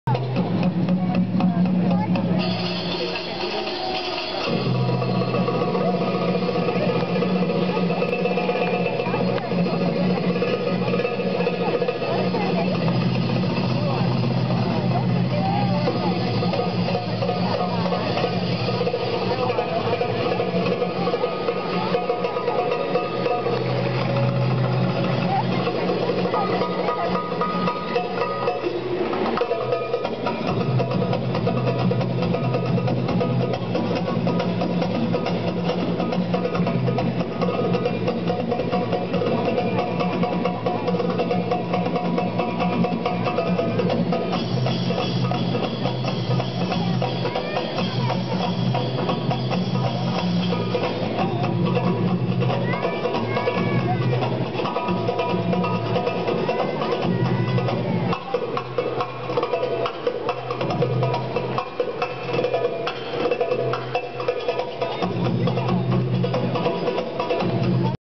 Bellydancer Nour Carassauga Mississauga 2009
Nour Hayati performs a drum solo at Carassauga to Hossam Ramzy